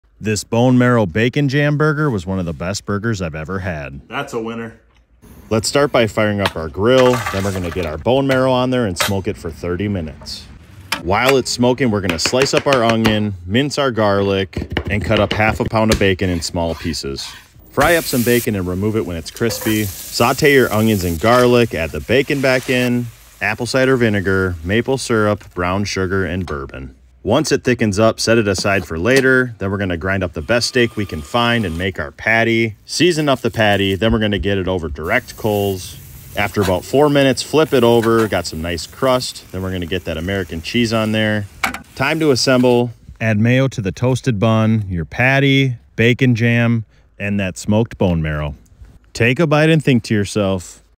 How to Make a Perfect Bone Marrow Bacon Jam Burger
This Bone Marrow Bacon Jam Burger by Nick Nesgoda is about to change your life 🤤🔥

Grill by @nomadgrills

Don't miss GrillWeek, now streaming on Tastemade! Catch your favorite shows all week long featuring everything sizzling, seared and charred to perfection.

Bacon Jam Recipe:

1. Slice up 1/2lb of bacon, cook until it starts to crisp up then remove bacon and leave 1 tbsp bacon fat and sauté 1/2 onion and 3 cloves of garlic for about 5 mins.

2. Add bacon back in, 1/4 cup apple cider vinegar, 1/4 cup maple syrup, 1/4 cup brown sugar, 2oz bourbon and cook for about 5 mins over med heat until it thickens

3. Bone marrow: Smoke for about 25mins until it starts to bubble

4. Grill the burger over direct coals, flip after 4 mins then add cheese. Assemble burger on a toasted brioche bun with Mayo,  bacon jam, smoked bone marrow